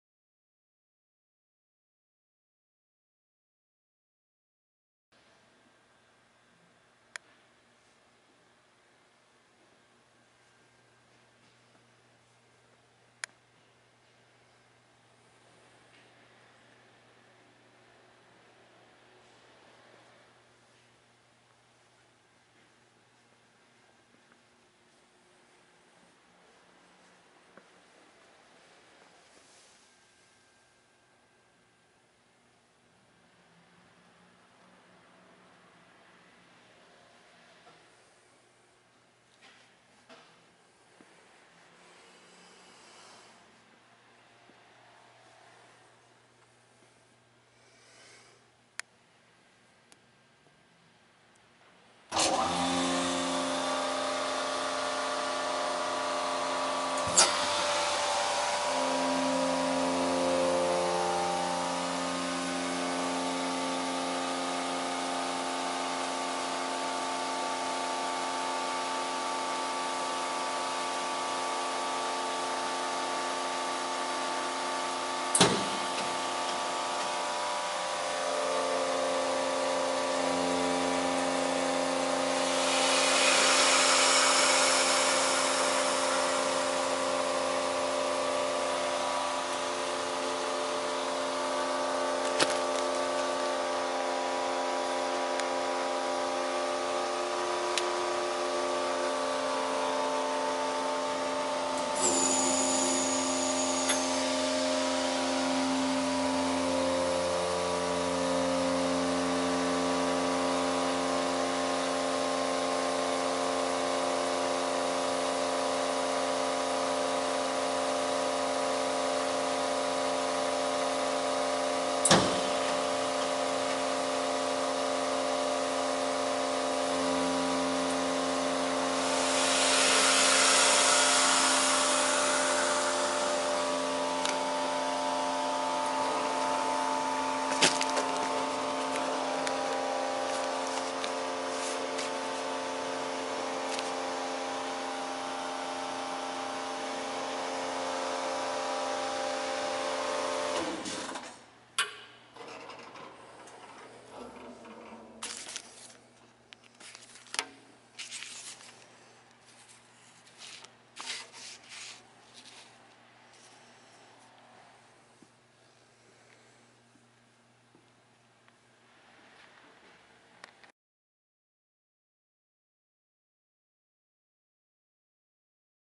H3558 Multivac C500 Dual Chamber Vacuum Bag Sealer SIGMA Equipment
This is a functional video of H3558 Multivac C500 Dual Chamber Vacuum Bag Sealer. for more available equipment.